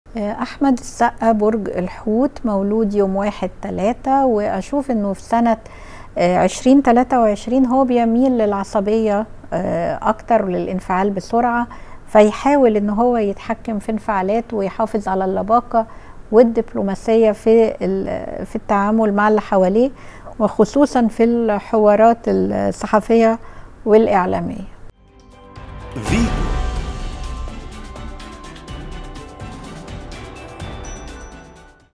توقعات أبراج 2023 لـ أحمد السقا: عليك الانتباه لهذه الأمور في العام الجديد
توقعات ابراج 2023.. كشفت خبيرة الأبراج وعلم الفلك عبير فؤاد عن الملامح الفلكية للعام الجديد، وأهم حظوظ الابراج في 2023، والابراج الاكثر حظا 2023، وتوقعات الابراج لسنة 2023، وأخطر أيام 2023.
ووجهت خبيرة الأبراج عدة نصائح للفنان أحمد السقا من أجل تجنب التحديات التي تواجهه في عام 2023، من أبرزها العصبية.